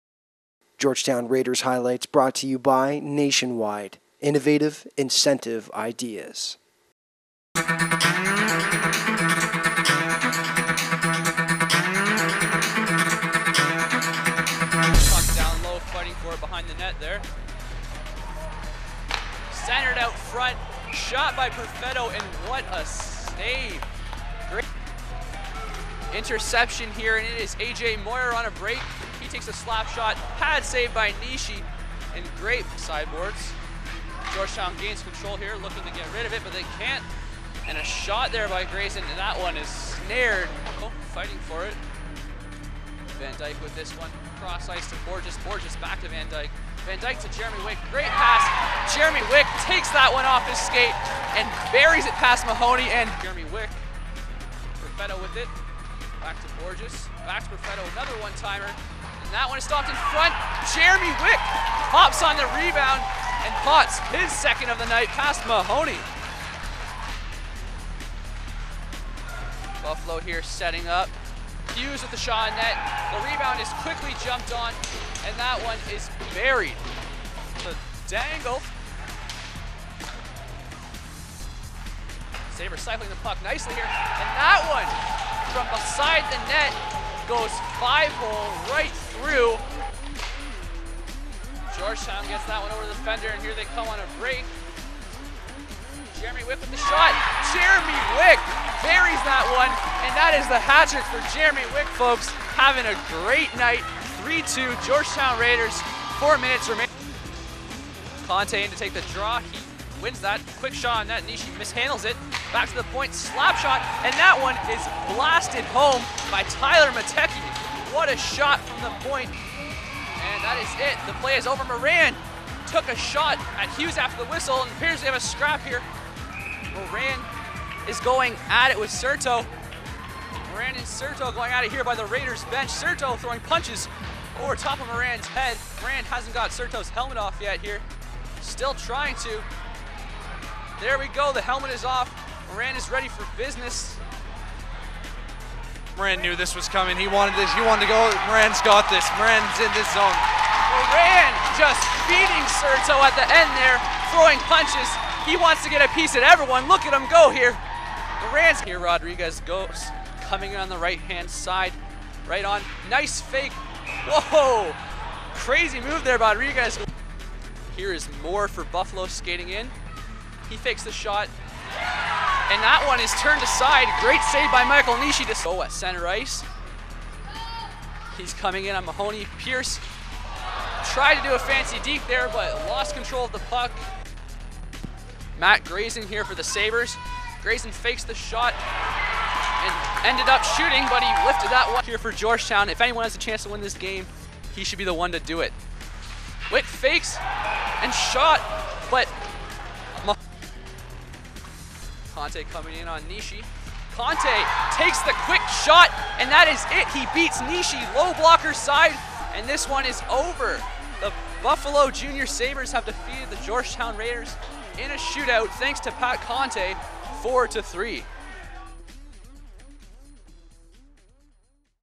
Georgetown Raiders vs. Buffalo Jr. Sabres (January 23, 2010)
An intense battle as the Raiders and Sabres square off in OJAHL action. Great scrap at the 2:05 mark.